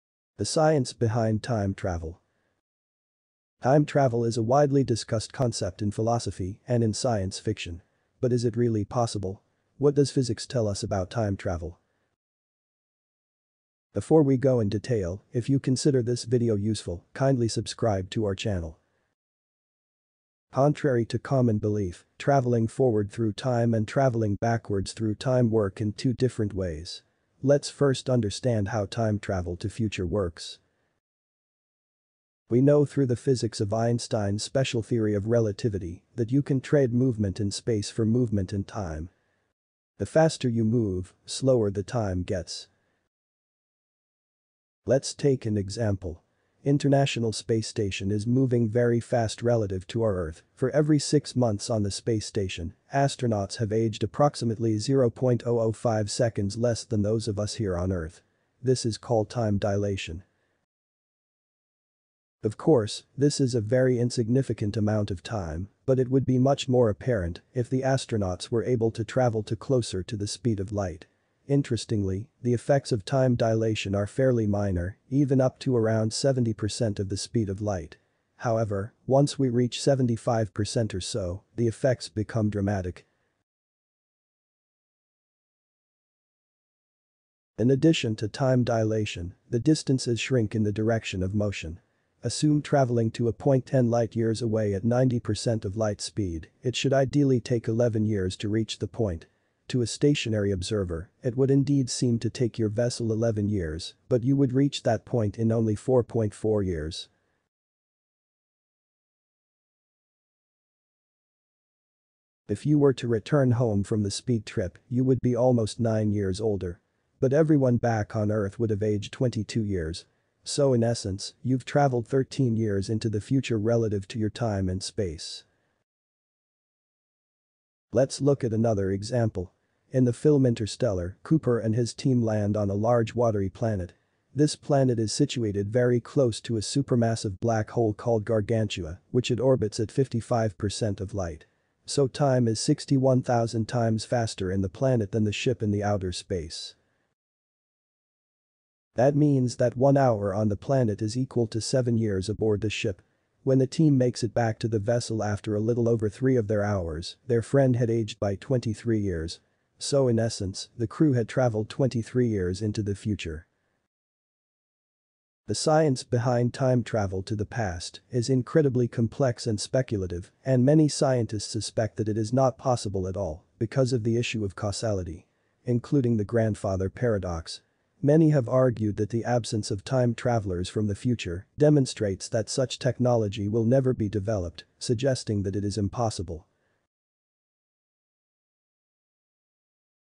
Is Time Travel Possible? Can we alter past?
Can we really travel through time? What is the Science behind time travel? What are the laws of time travel? In the video we explain all you need to know to understand time travel.

Is it possible to build a time machine?
Travelling in time might sound like a flight of fancy, but some physicists think it might really be possible. But its not how its shown in movies.

s it theoretically possible to travel forward in time?
While the debate continues over whether travelling into the past is possible, physicists have determined that travelling to the future most certainly is. And you don't need a wormhole or a DeLorean to do it. Real-life time travel occurs through time dilation, a property of Einstein's special relativity.

Albert Einstein came up with an idea about how time works. He called it relativity. This theory says that time and space are linked together. Well, according to this theory, the faster you travel, the slower you experience time. Scientists have done some experiments to show that this is true.

For example, astronauts who move very fast experience a very small time dilation.

Does the future already exist?
The past and future do not exist and are only concepts used to describe the real, isolated, and changing present. This conventional model presents a number of difficult philosophical problems, and seems difficult to reconcile with currently accepted scientific theories such as the theory of relativity.

Is it possible to go back in time and change the past?
Time travellers cannot change the past: they cannot make it different from the way it was—but they can participate in it: they can be amongst the people who did make the past the way it was.

But what is the exact science? Can we travel back in time to the past? Is time travel a one way ticket?